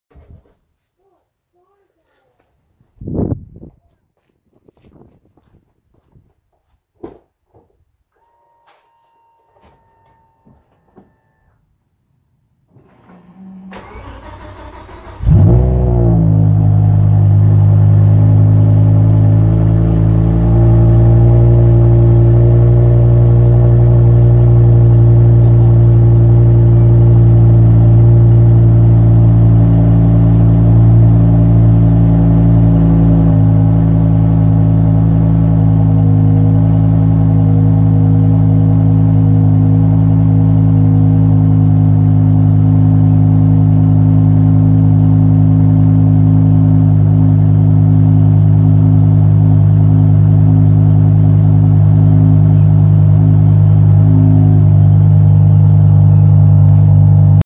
BMW E60 M5 cold start
Cold start on my M5 with newly installed Evil Energy 2.5" remote exhaust cutouts.  The noise was incredible and I couldn't help but laugh.  Great mod for under $300 installed.